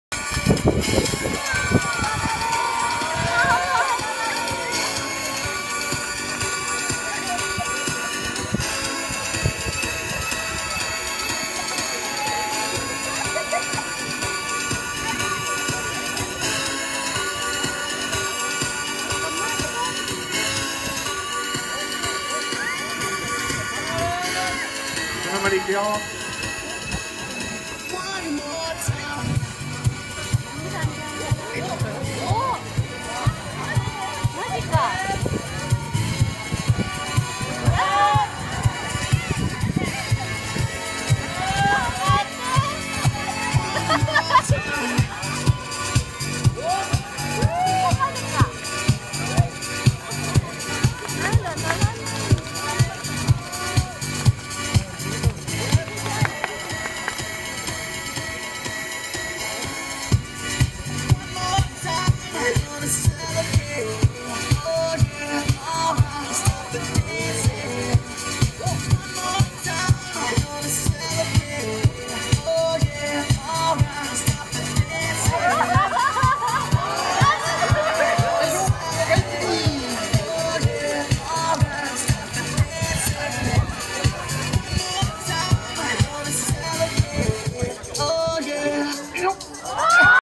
スカイツリーやってみた♪( ´▽｀)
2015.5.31ご当地キャラクターフェスすみだ
SANOMARU does "TOKYO SKY TREE" !